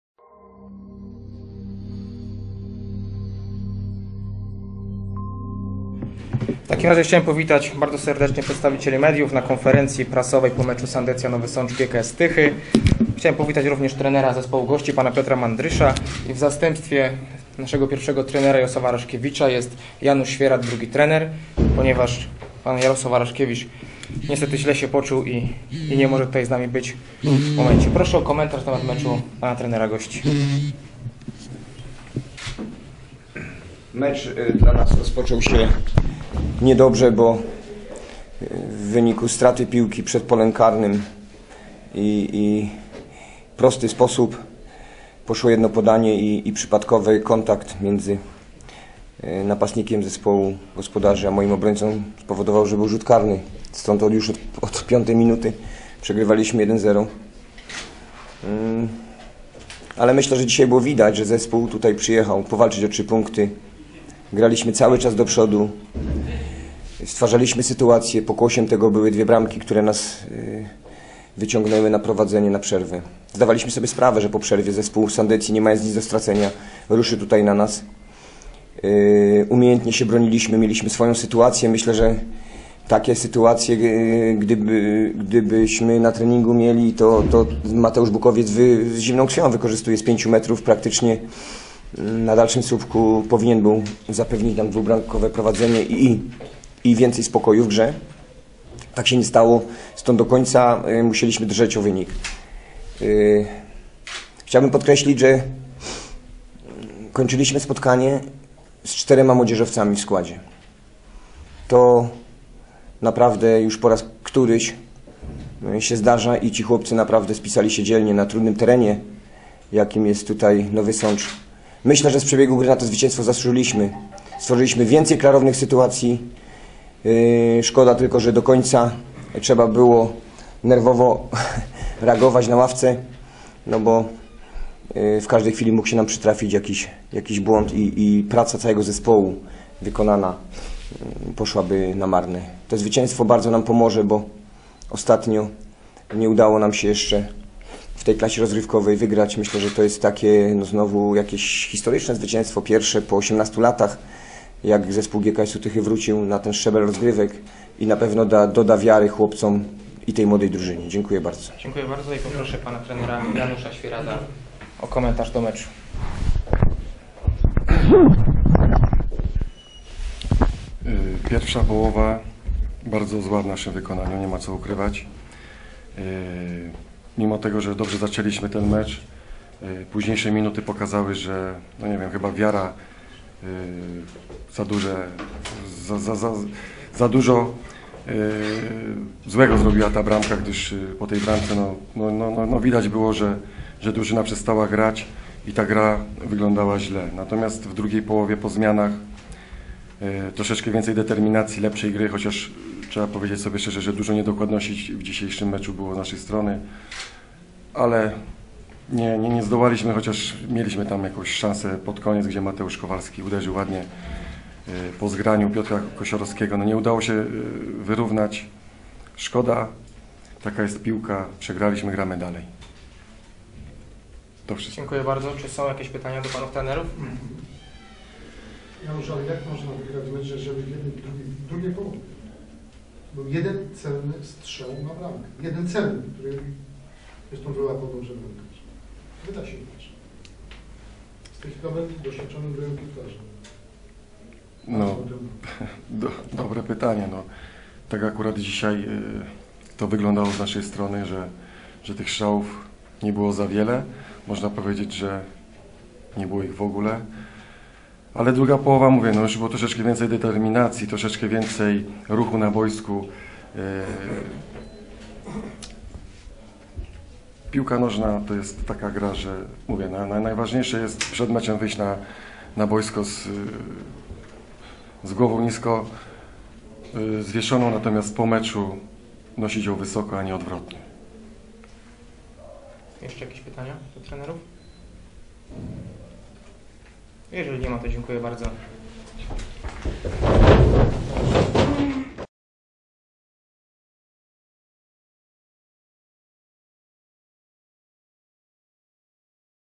6. kolejka: Sandecja Nowy Sącz - GKS Tychy 1:2. Pomeczowa konferencja
Konferencja prasowa po meczu Sandecja Nowy Sącz - GKS Tychy, z udziałem trenerów Piotra Mandrysza i Janusza Świerada.